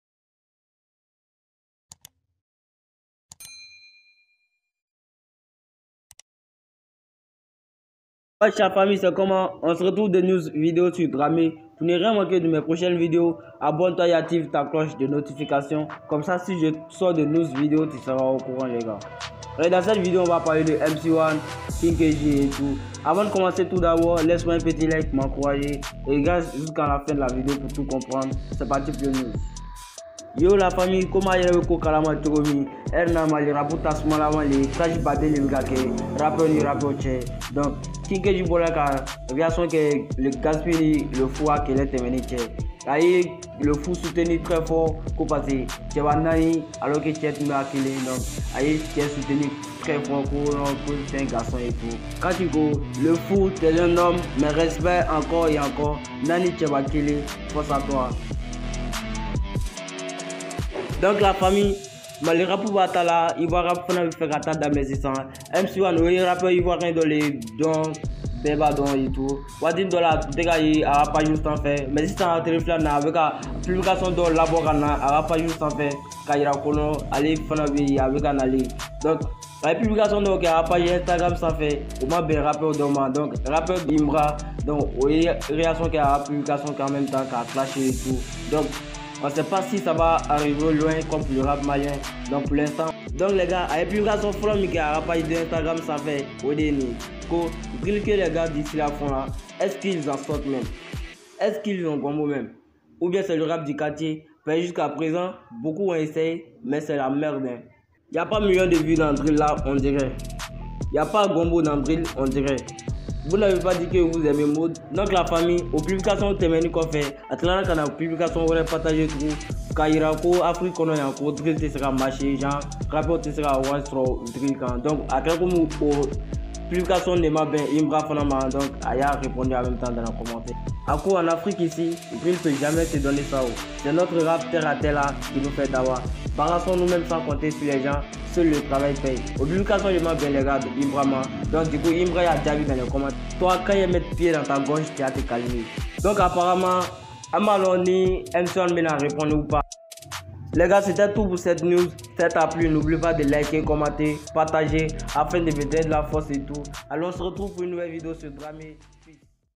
Iba one, KiNG Kj réagis au bagarre de Gasp! & Le Fou || mc one clashé par Himra
met ton avis en commentaire 📝

besoin de collaboration
(part, pub,promo,...)

contacter : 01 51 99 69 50